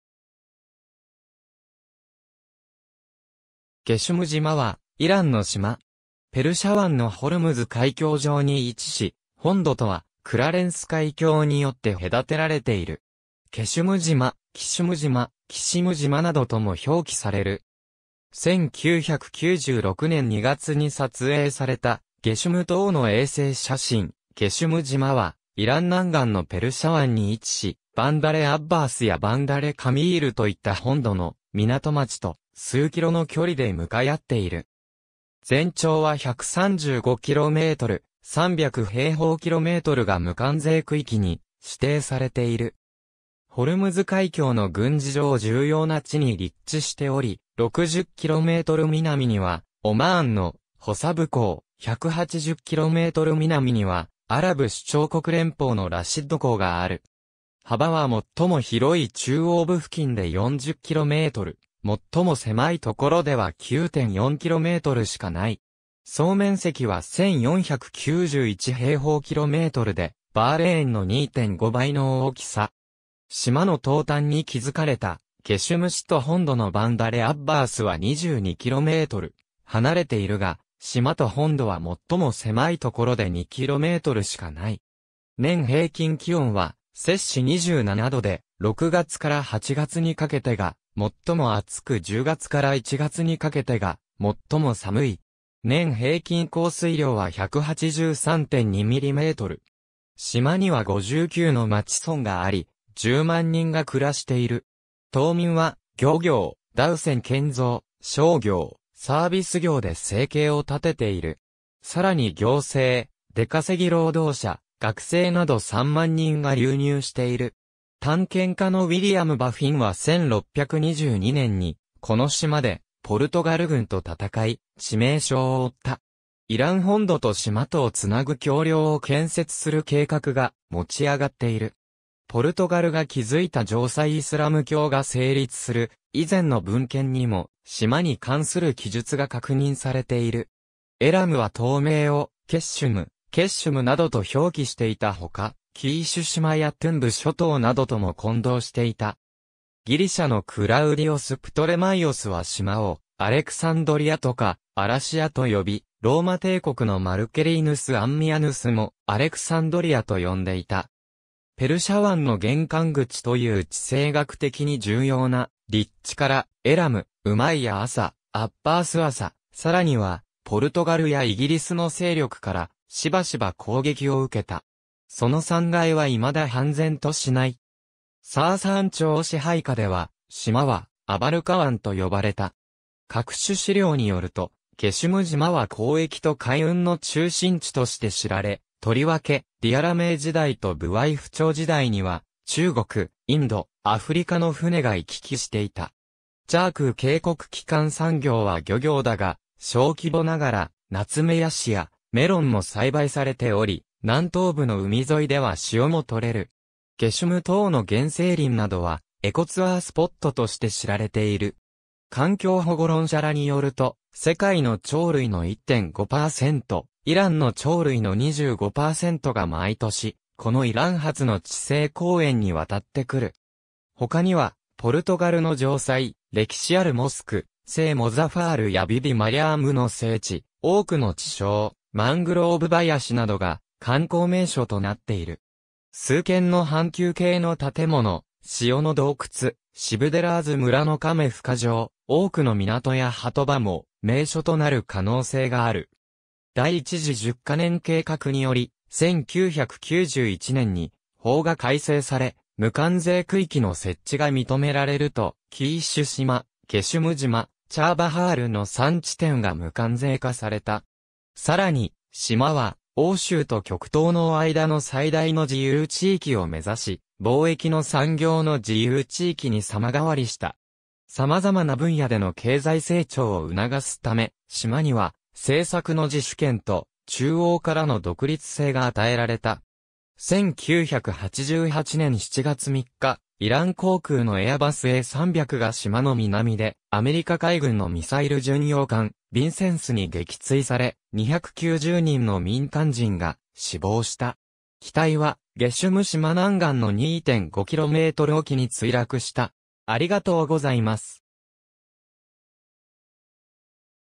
ゲシュム島
ゲシュム島（ペルシア語: قشم‎、英: Qeshm）は、イランの島。
ペルシャ湾のホルムズ海峡上に位置し、本土とはクラレンス海峡によって隔てられている。
ケシュム島、キシュム島、キシム島などとも表記される。
1996年2月に撮影されたゲシュム島の衛星写真 ゲシュム島はイラン南岸のペルシャ湾に位置し、バンダレ・アッバースやバンダレ・カミールといった本土の港町と数キロの距離で向かい合っている。
全長は135km、300平方キロメートルが無関税区域に指定されている。
ホルムズ海峡の軍事上重要な地に立地しており、60km南にはオマーンのホサブ港、180km南にはアラブ首長国連邦（UAE）のラシッド港がある。
幅は最も広い中央部付近で40km、最も狭いところでは9.4kmしかない。
総面積は1491平方キロメートルで、バーレーンの2.5倍の大きさ。
島の東端に築かれたゲシュム市と本土のバンダレ・アッバースは22km離れているが、島と本土は最も狭いところで2kmしかない。
年平均気温は27℃で、6月から8月にかけてが最も暑く10月から1月にかけてが最も寒い。
年平均降水量は183.2mm。
島には59の町村があり、10万人が暮らしている。
島民は漁業、ダウ船建造、商業、サービス業で生計を立てている。
さらに行政、出稼ぎ労働者、学生など3万人が流入している。
探検家のウィリアム・バフィンは1622年にこの島でポルトガル軍と戦い、致命傷を負った。
イラン本土と島とをつなぐ橋梁を建設する計画が持ち上がっている。
ポルトガルが築いた城塞 イスラム教が成立する以前の文献にも、島に関する記述が確認されている。
エラムは島名をQeshm、Keshmなどと表記していたほか、キーシュ島やトゥンブ諸島などとも混同していた。
ギリシャのクラウディオス・プトレマイオスは島をアレクサンドリアとかアラシアと呼び、ローマ帝国のマルケリヌス・アンミアヌスもアレクサンドリアと呼んでいた。
ペルシャ湾の玄関口という地政学的に重要な立地から、エラム、ウマイヤ朝、アッバース朝、さらにはポルトガルやイギリスの勢力からしばしば攻撃を受けた。
その惨害は未だ判然としない。
サーサーン朝支配下では、島はアバルカワン（Abarkawan）と呼ばれた。
各種史料によると、ゲシュム島は交易と海運の中心地として知られ、とりわけディアラメー時代とブワイフ朝時代には中国、インド、アフリカの船が行き来していた。
チャークー渓谷 基幹産業は漁業だが、小規模ながらナツメヤシやメロンも栽培されており、南東部の海沿いでは塩もとれる。
ゲシュム島の原生林などはエコツアースポットとして知られている。
環境保護論者らによると、世界の鳥類の1.5%、イランの鳥類の25%が毎年、このイラン初の地勢公園に渡ってくる。
他にはポルトガルの城塞、歴史あるモスク、セイ・モザファールやビビ・マリャームの聖地、多くの池沼、マングローブ林などが観光名所となっている。
数軒の半球形の建物、塩の洞窟、シブデラーズ村のカメ孵化場、多くの港や波止場も名所となる可能性がある。
第一次10ヵ年計画により、1991年に法が改正され無関税区域の設置が認められると、キーシュ島、ゲシュム島、チャーバハールの三地点が無関税化された。
さらに、島は欧州と極東の間の最大の自由地域を目指し「貿易の産業の自由地域」に様変わりした。
様々な分野での経済成長を促すため、島には政策の自主権と中央からの独立性が与えられた。
1988年7月3日、イラン航空のエアバスA300が島の南でアメリカ海軍のミサイル巡洋艦、ヴィンセンスに撃墜され、290人の民間人が死亡した（イラン航空655便撃墜事件）。
機体はゲシュム島南岸の2.5km沖に墜落した。